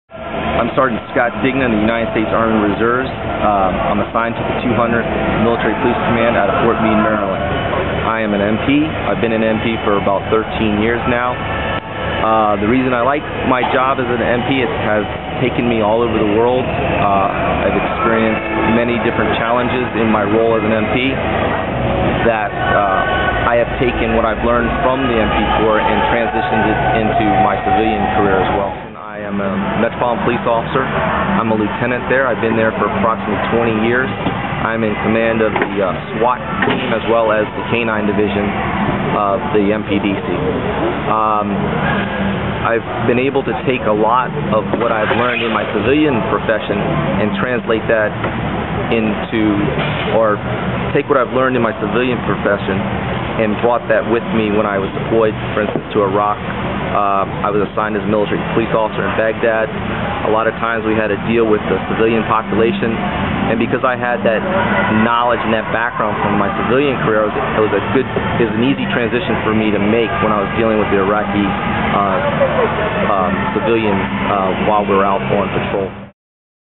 Interview: Sgt. Dignan
ARMY.MIL - At the Joint Services Open House on Andrews Air Force Base, Sgt. Scott Dignan, 200th MP Command, Army Reserves, talks about his role as an MP out of Fort Meade, and how it relates to his civilian job as a Lieutenant Metropolitan Police Officer.